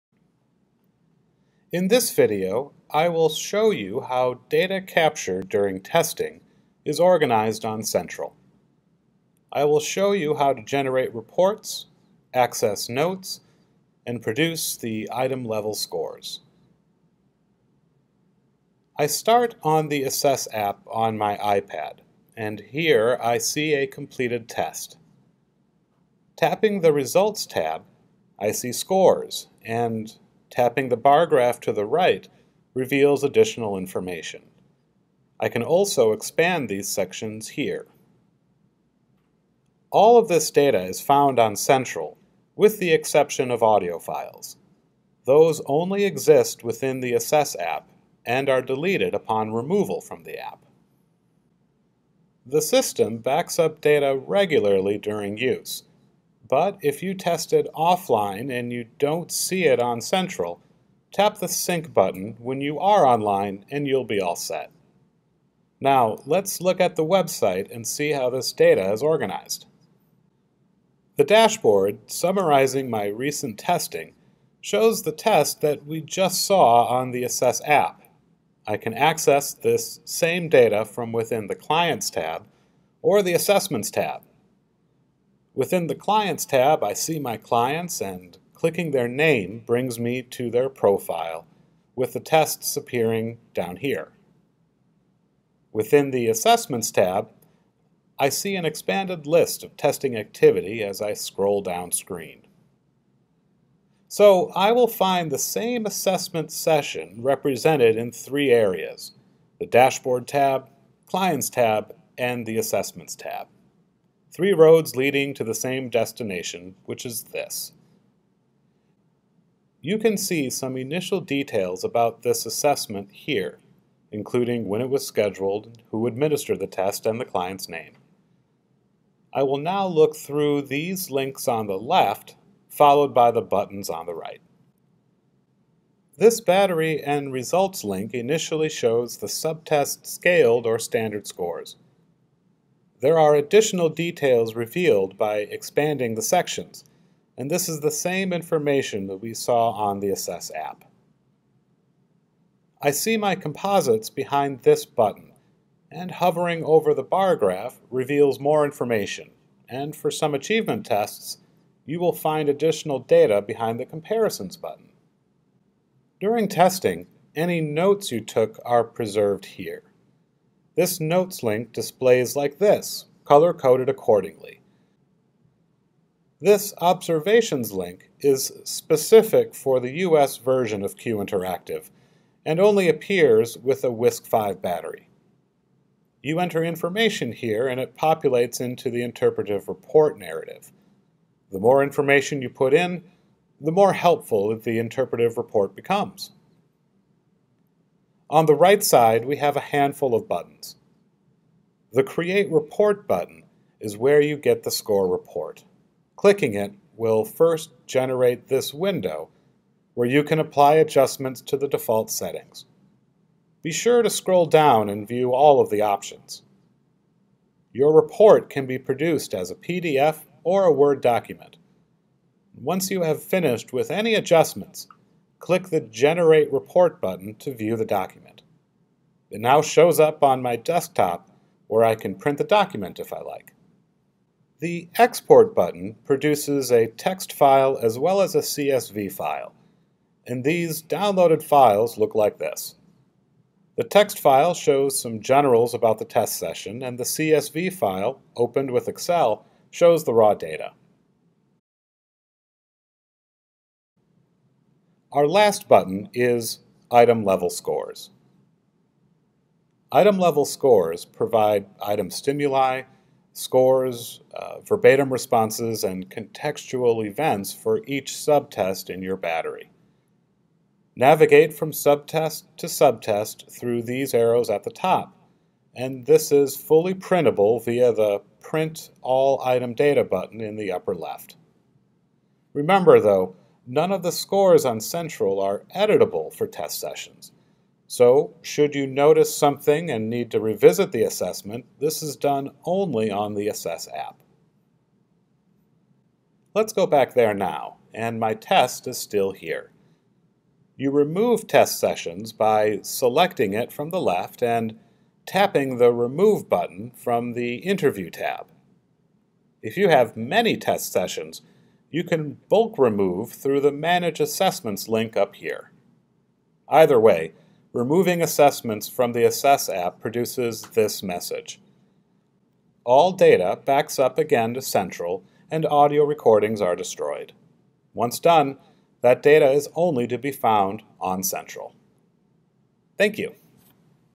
Reporting
A walkthrough of how data captured during testing is organized on Central.  We show how reports are generated and customized, how item-level scores are accessed and printed, and how observational notes captured during testing are available on Central.